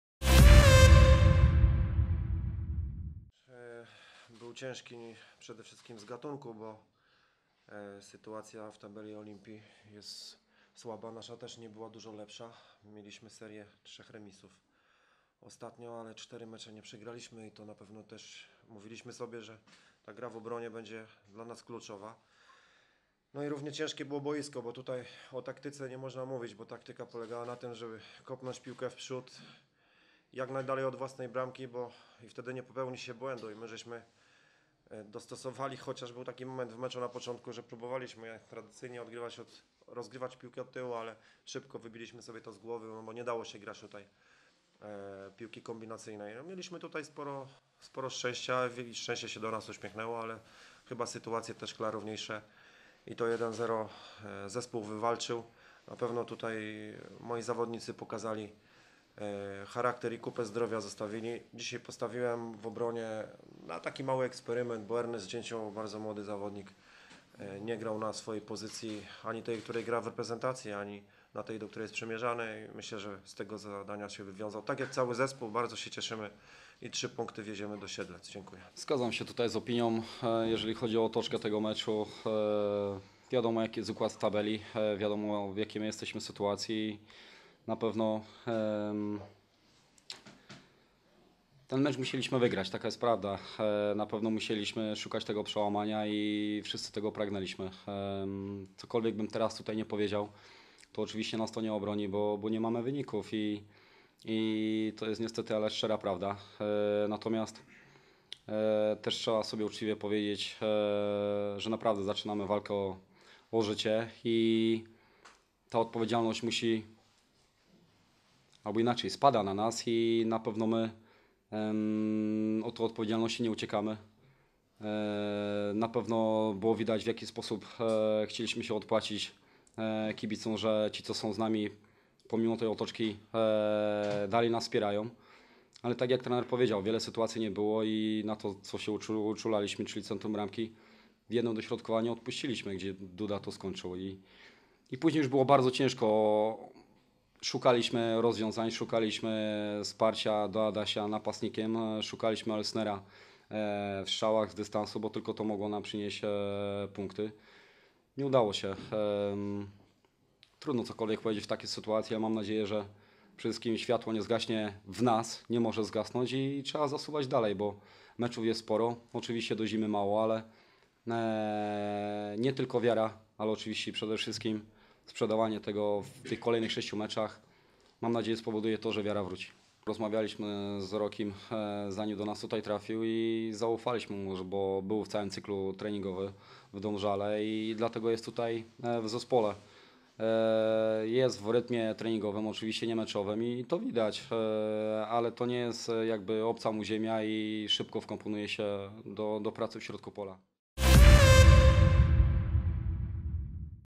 Konferencja prasowa po meczu Olimpia Grudziądz - Pogoń Siedlce (0:1)
Gośćmi konferencji byli:
Marcin Sasal - trener Pogoni Siedlce
Artur Skowronek - trener Olimpii Grudziądz